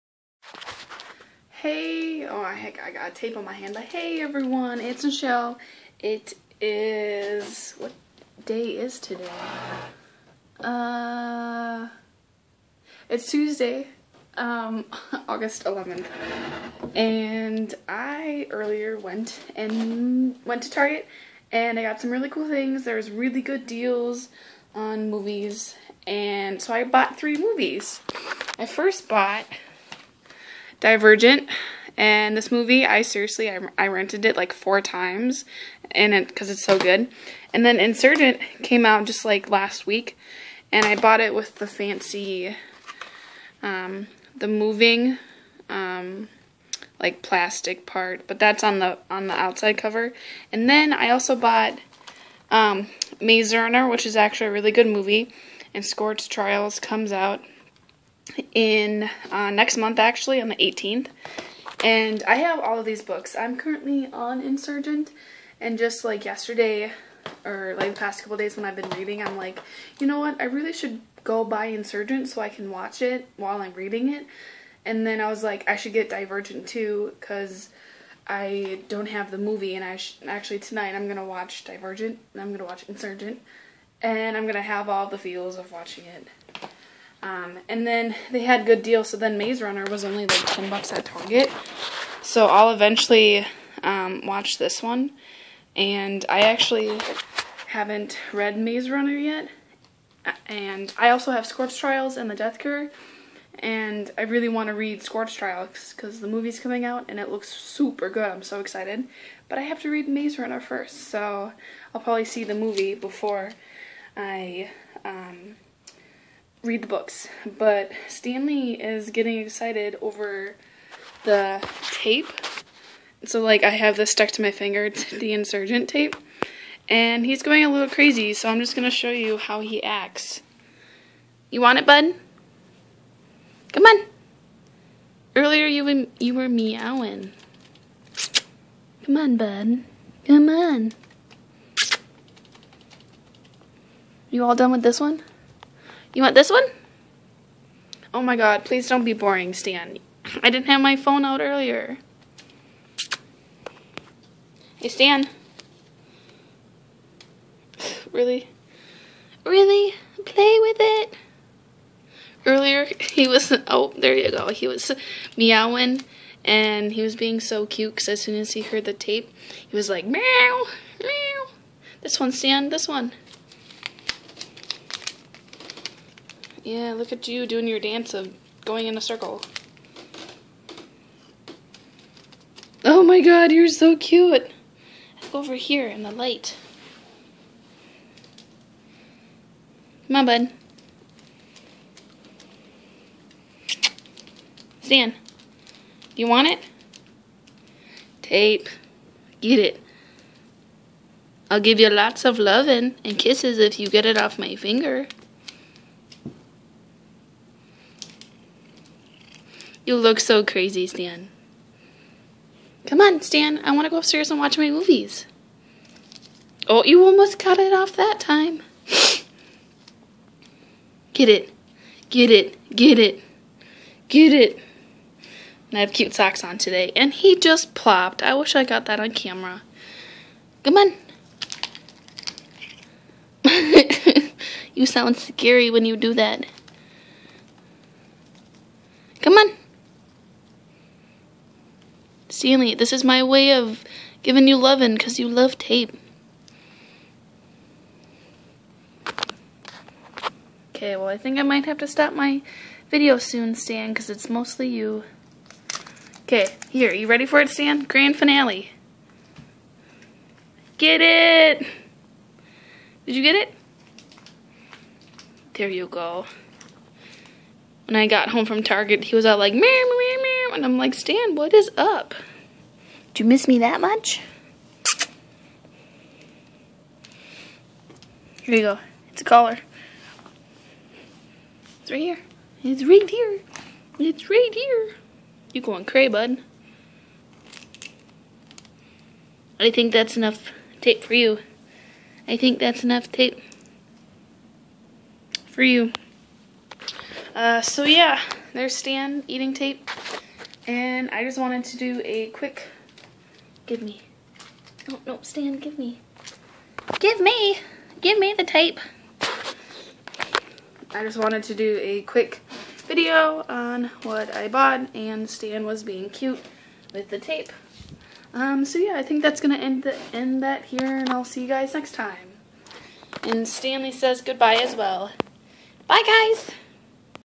MOVIES HAUL & STANLEY'S TAPE ANTICS // AUGUST 11TH VLOG
I show you guys the movies I bought just before I did this vlog, which include my favorite from the Divergent and The Maze Runner series!! And Stanley being his typical self once he hears the sound of tape, it's free entertainment for myself. :D